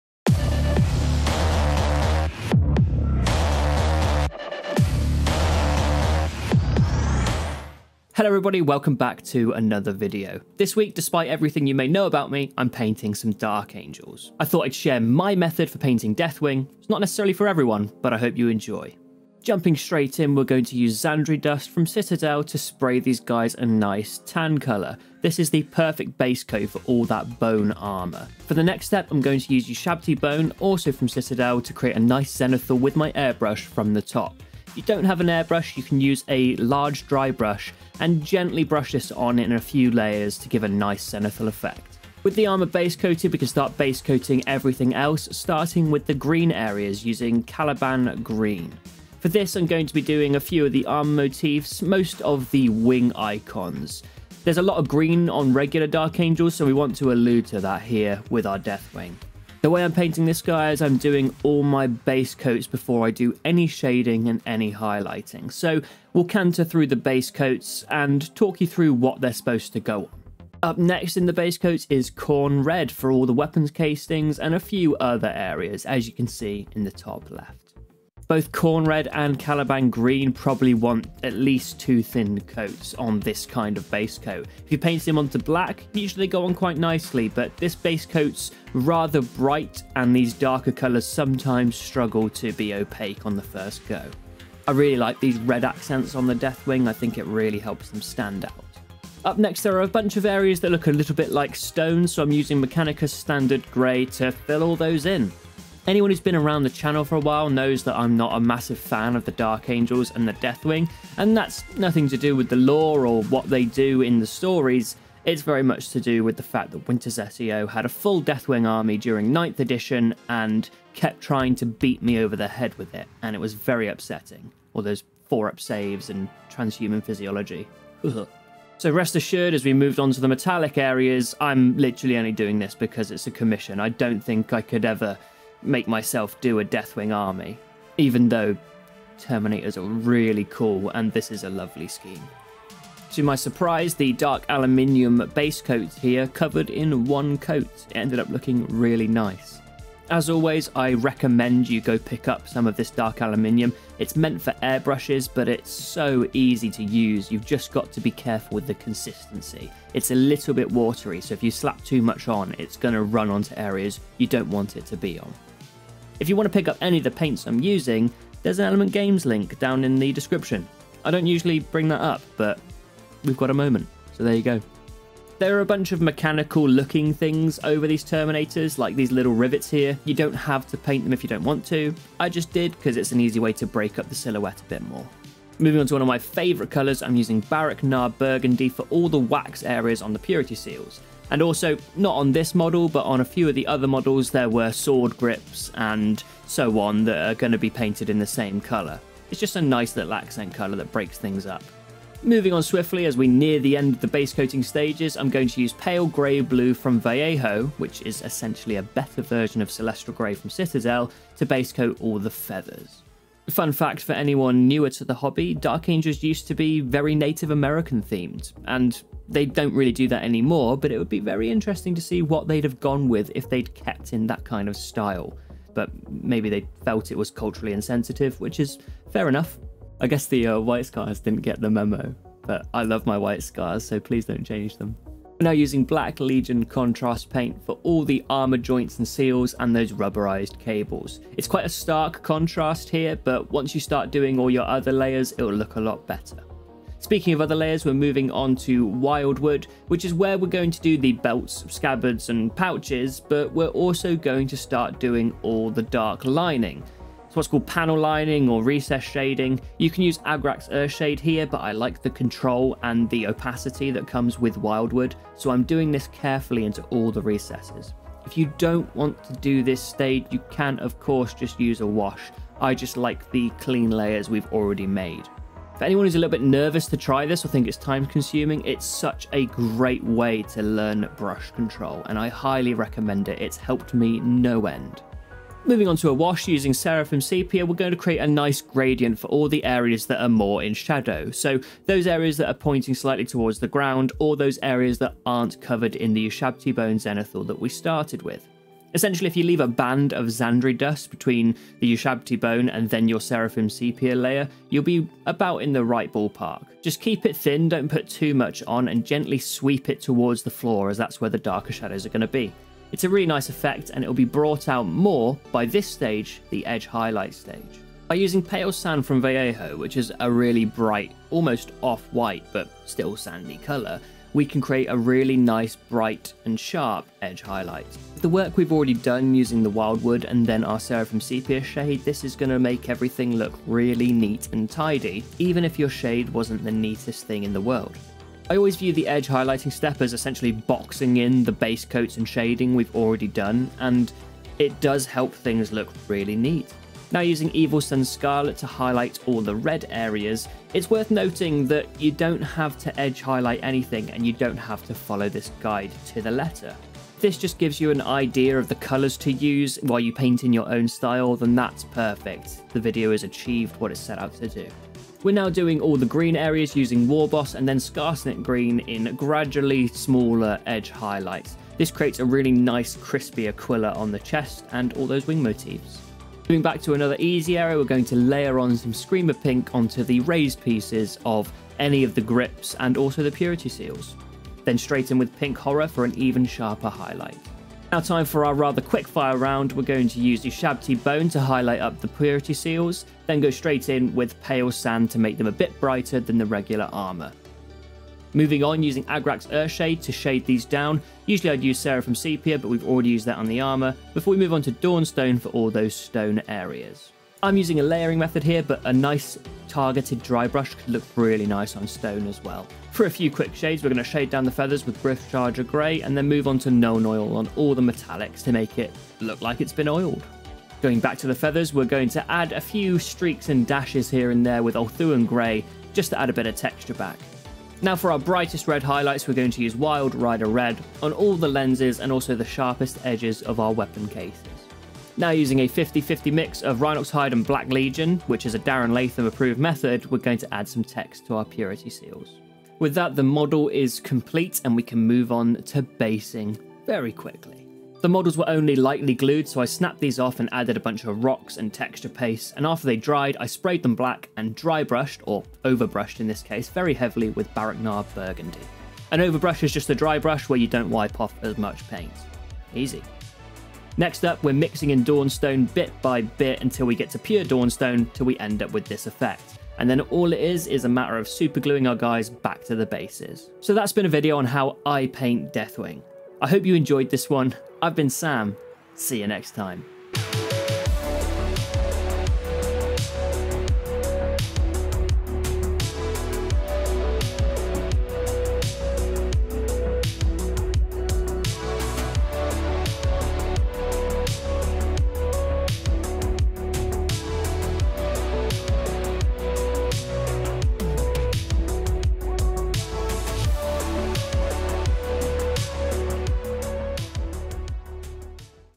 DEATHWING - The Crispiest Painting Method!
This week I paint up not only more Space Marines, but their from one of my most hated 9th edition Warhammer armies - Dark Angels! :o